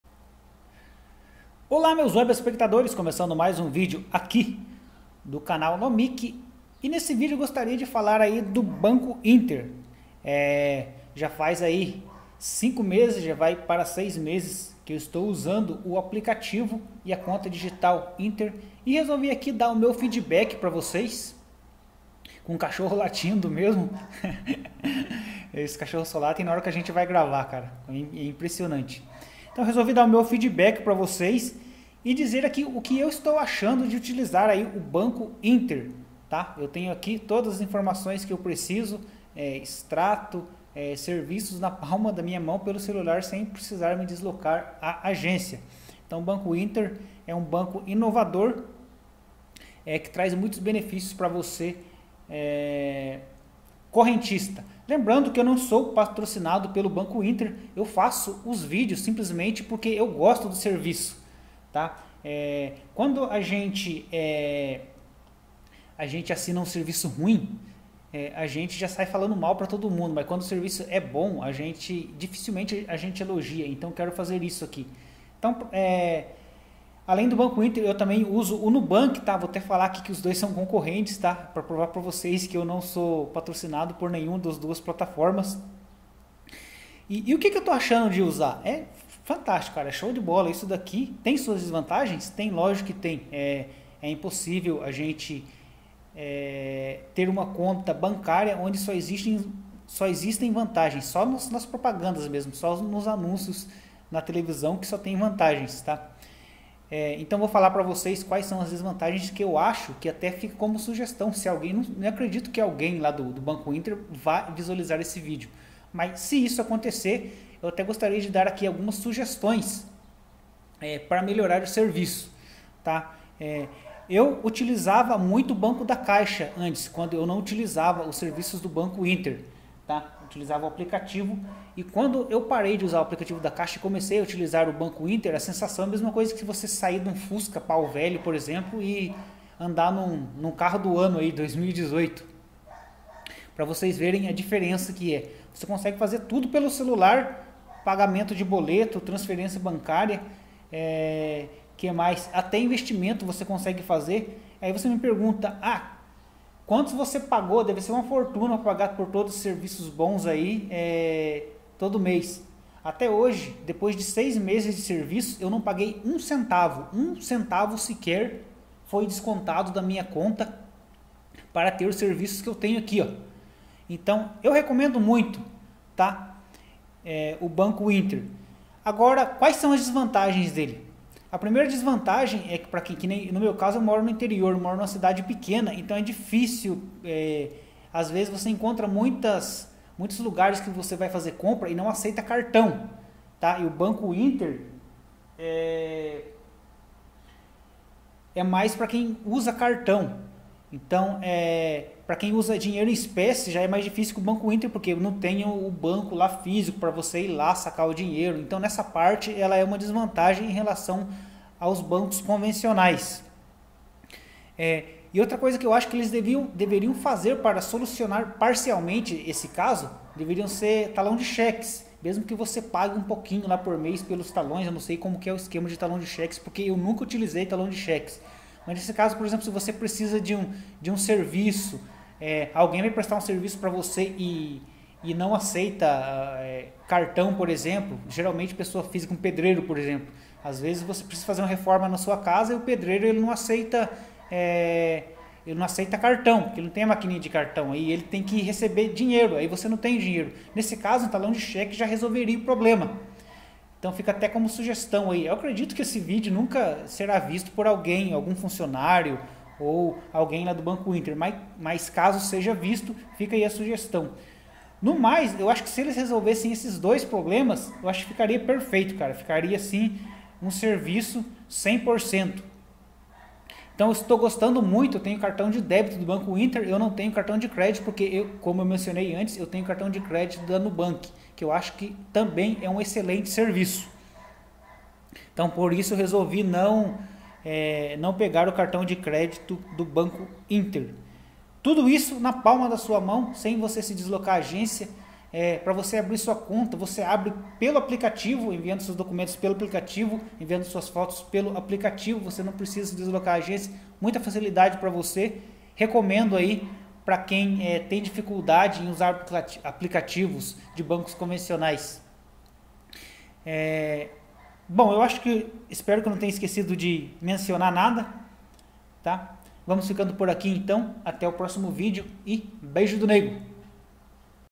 6 meses usando o BANCO INTER! O que estou achando?
Neste vídeos resolvi dar o meu feedback em relação ao aplicativo e a conta digital do banco Inter.

Já fez seis meses que estou utilizando o serviço e aqui eu comento algumas das vantagens e desvantagens deste serviço. Espero que gostem do vídeo.